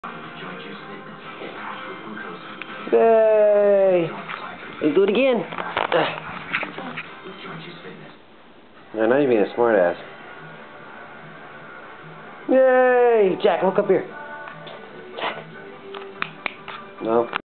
standing up
Jack standing up by himself